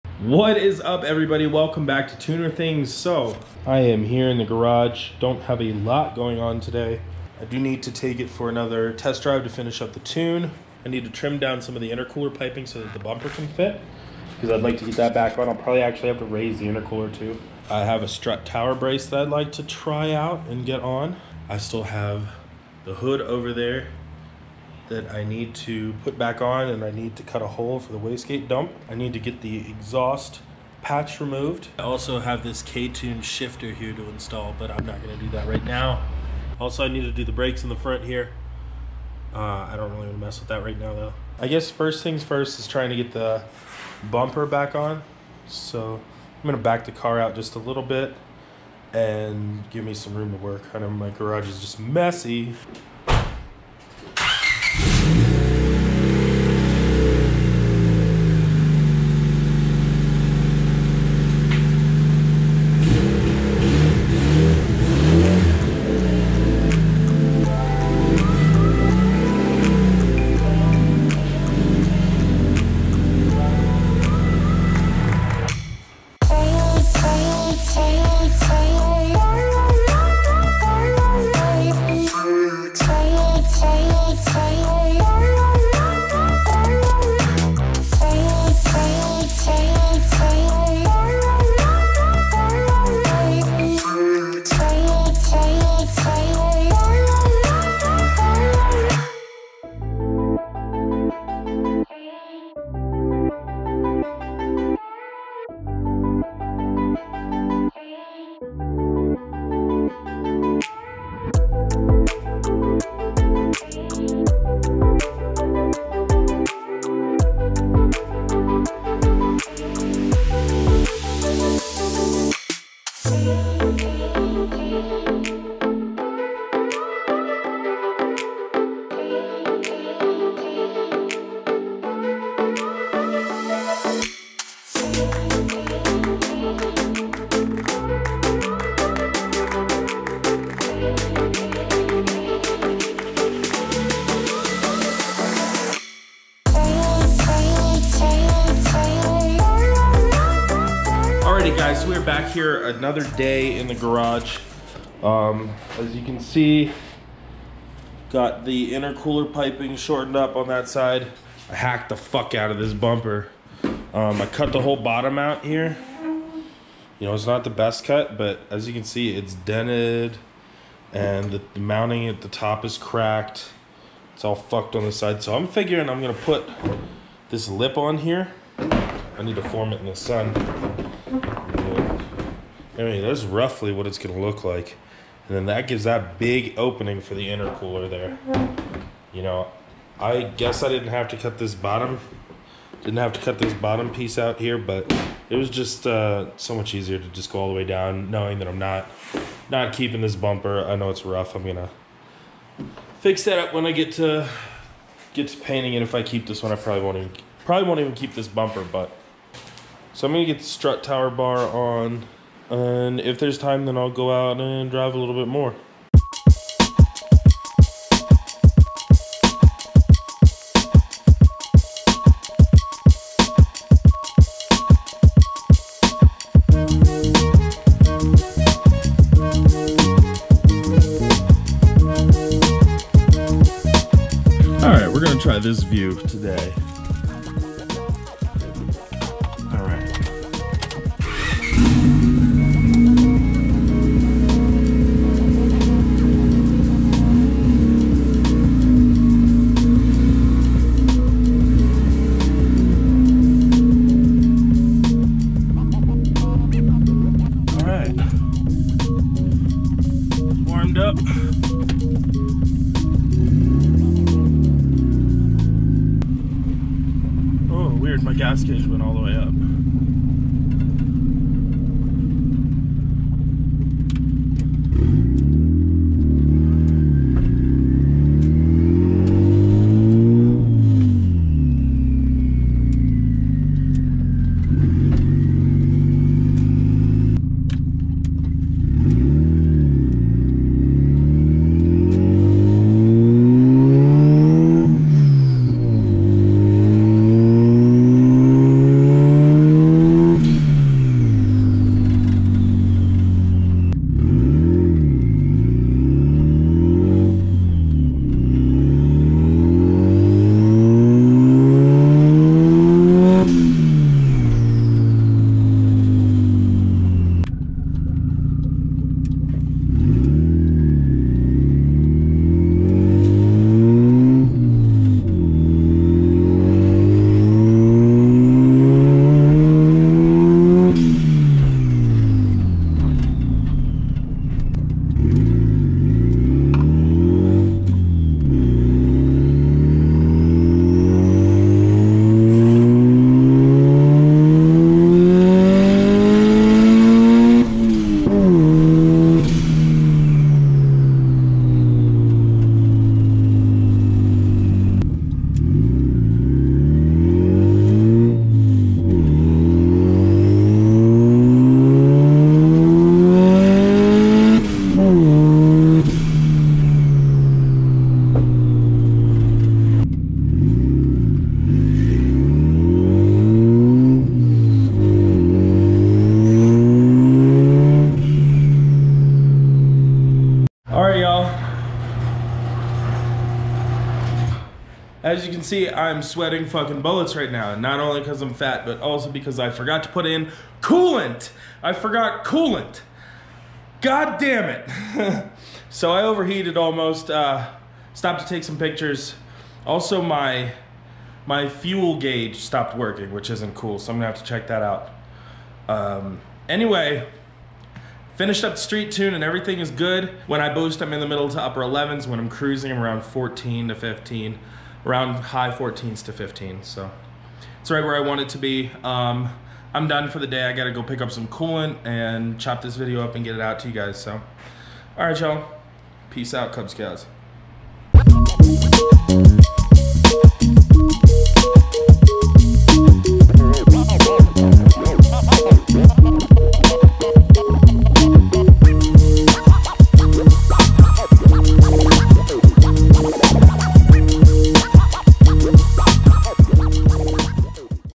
Finishing Turbo D16 Civic Street Tuning | TunerThings
Finishing Turbo D16 Civic Street Tuning

Got a lot of stuff done on the Turbo D16Y8 Civic over the last few days...

I got the intercooler piping shortened. I disconnected my exhaust patch because it was hanging down too low, and I installed my strut tower brace!

I also finished my street tune -- right now I'm pushing 11psi with air fuel ratios in the mid to high 11s when boosting and high 14s low 15s while cruising and under light acceleration.

The civic is tuned with Crome and a p28 ecu using a moates ostrich 2.0 to interface the ECU to my macbook.